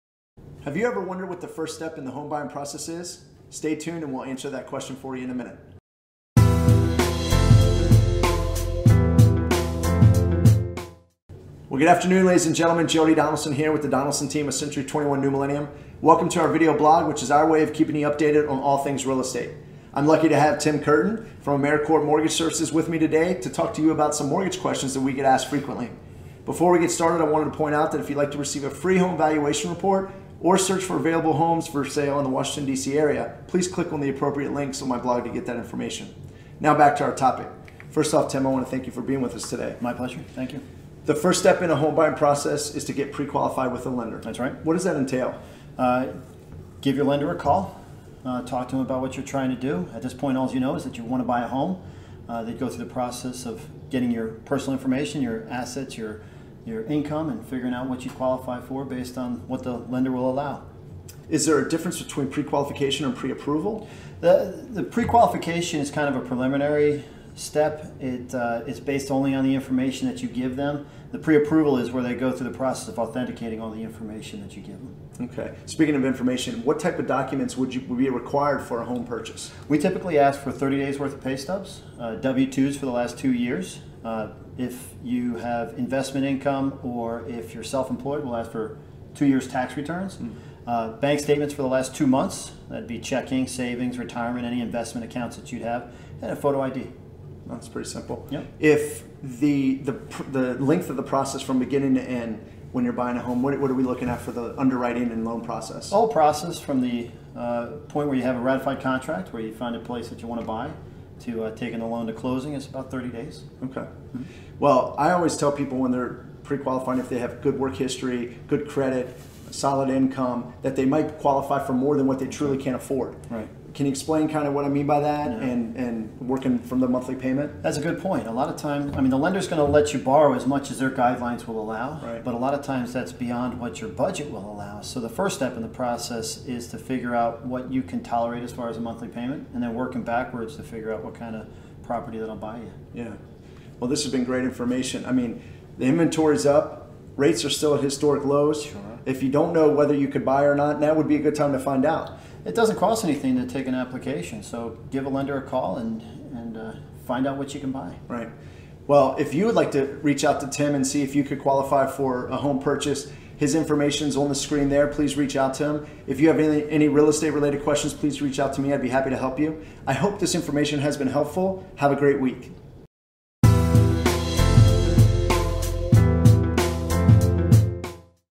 Northern Virginia Real Estate Agent: Begin the home buying process
Today, Tim Curtin from Americorp Mortgage Services joins me to discuss the very first step in the home buying process: getting pre-qualified. A pre-qualification is crucial. Before you even start looking at homes, you need to know which properties you can actually afford. To learn more, watch this short video!

Selling your Virginia home?

Buying a Virginia home? Search all homes for sale

Jody Donaldson
Century 21 New Millennium
4315 Walney Road